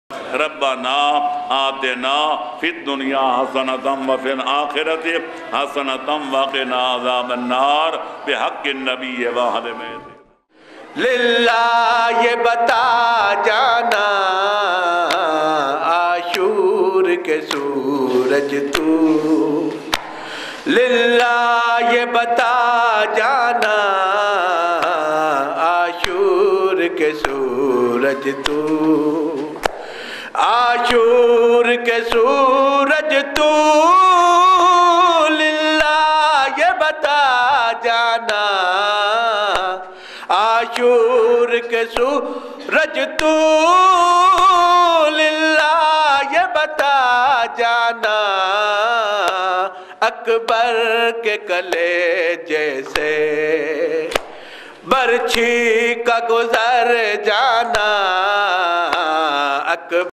Matam & Azadari by Ayatollah Muhammad Hussain Najafi Dhakku (8th Muharram 1435 / 2013, Sargodha)
Sargodha - 8th Muharram 1435 AH / 11th Nov. 2013 AD